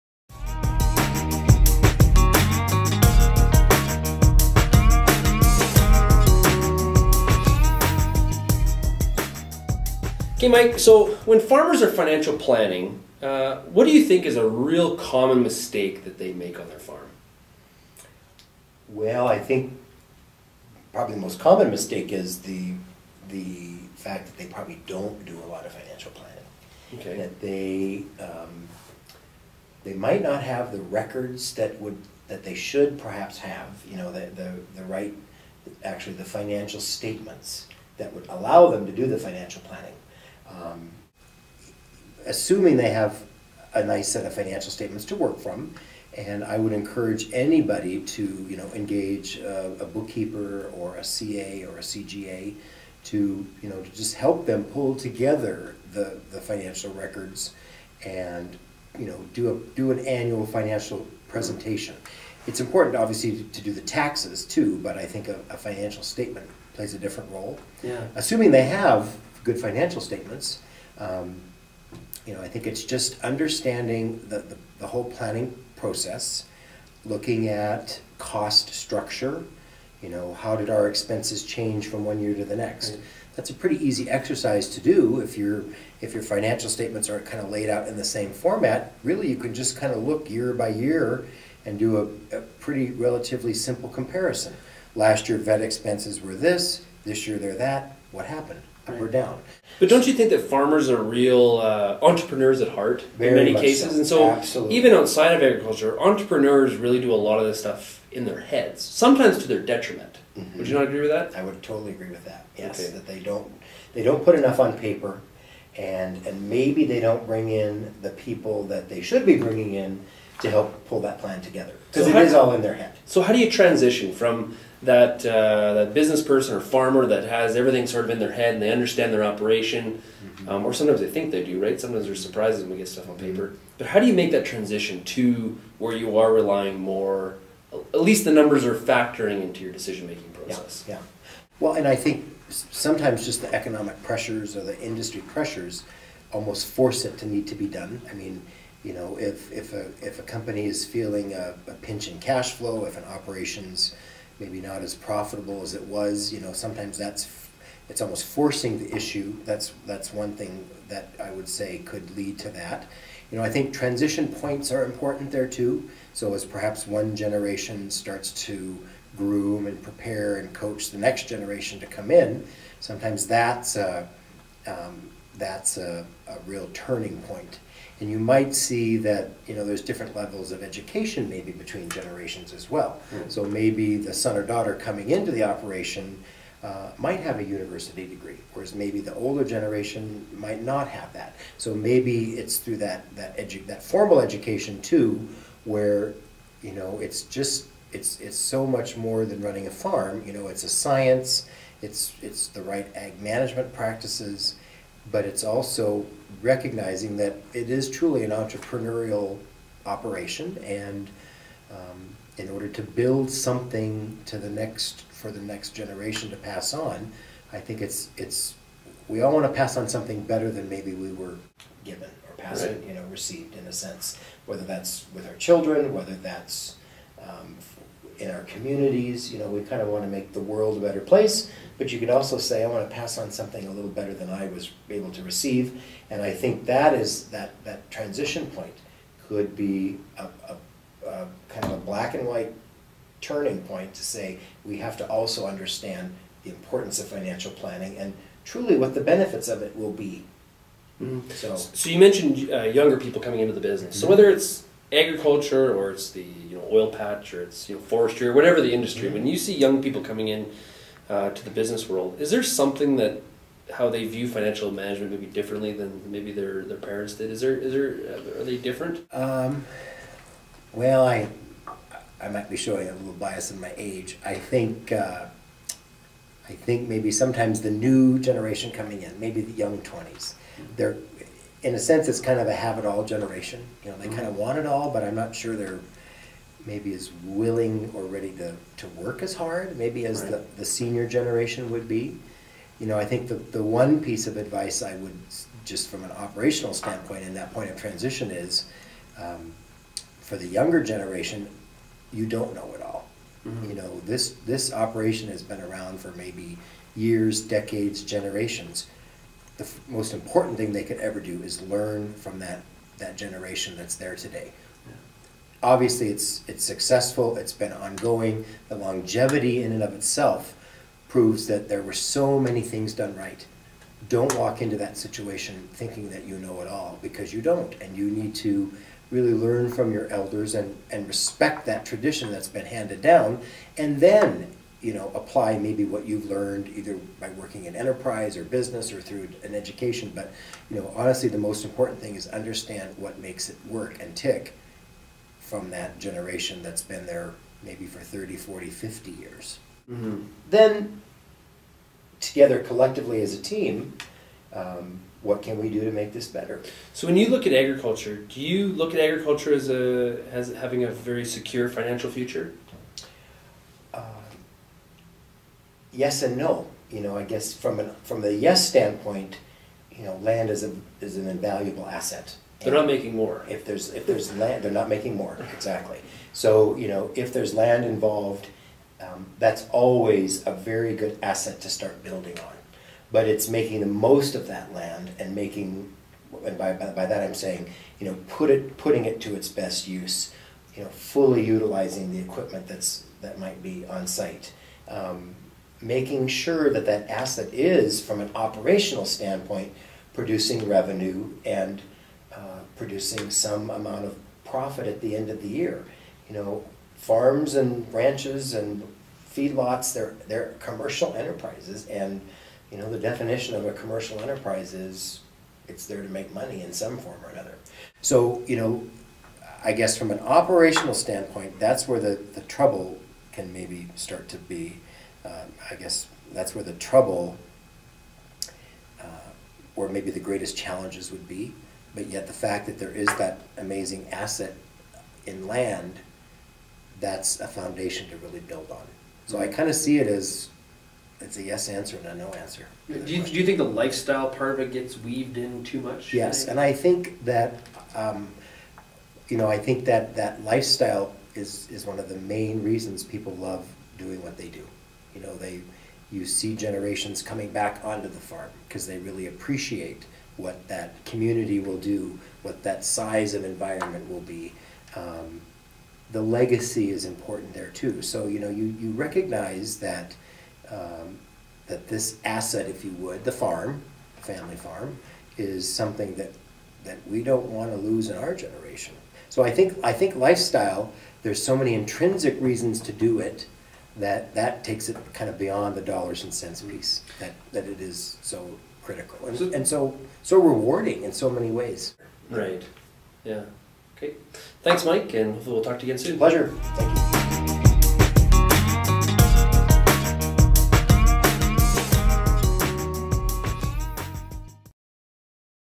Tiffin 2011 Mike Whitlock on Farm Financial Planning.
RealAgriculture.com discusses the necessity of a farm business plan with Tannian Consulting's Mike Whitlock